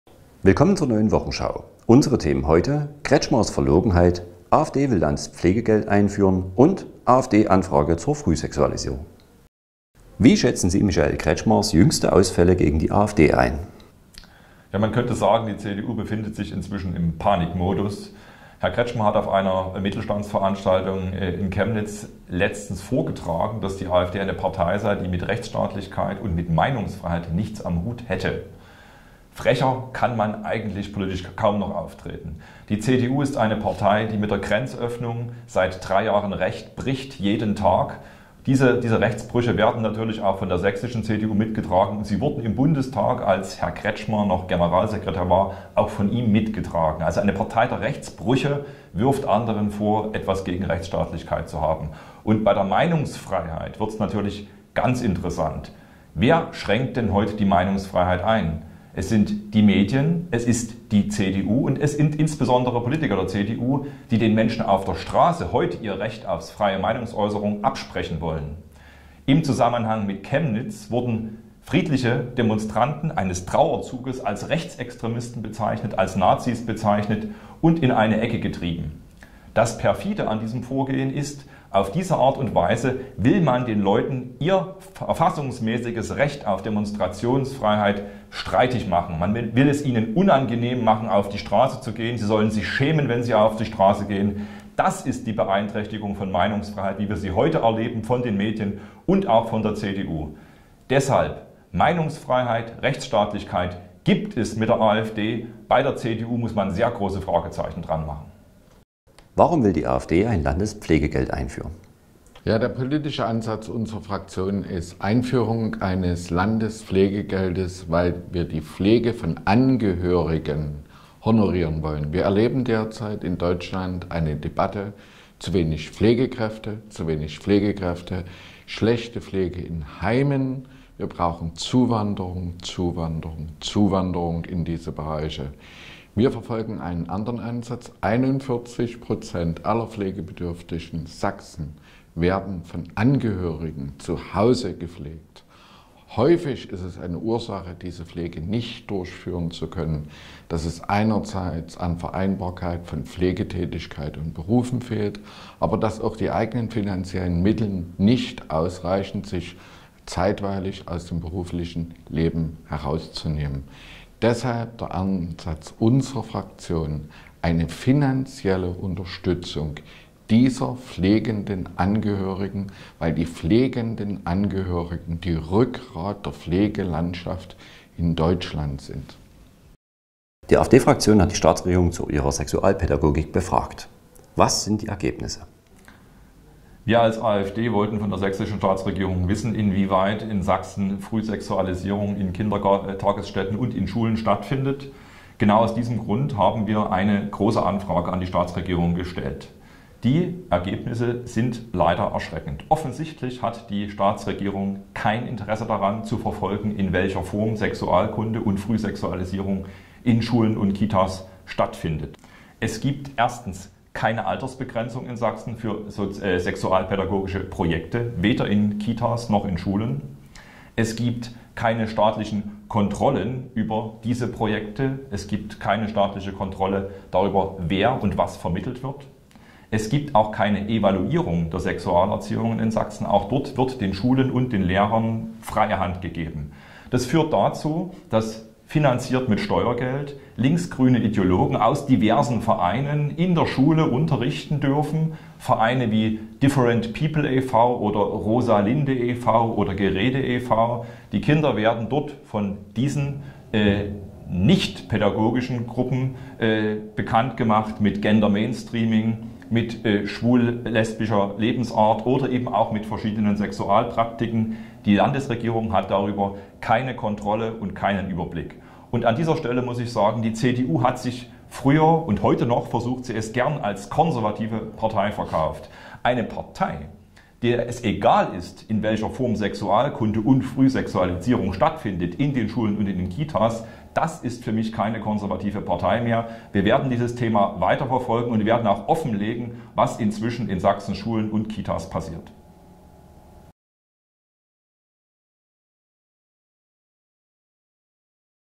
Blaue Wochenschau 28.09.2018
Themen: CDU-Ministerpräsident Kretschmers Verlogenheit, AfD-Antrag für Landes-Pflegegeld und AfD-Anfrage deckt Frühsexualisierung bereits in der Kinderkrippe auf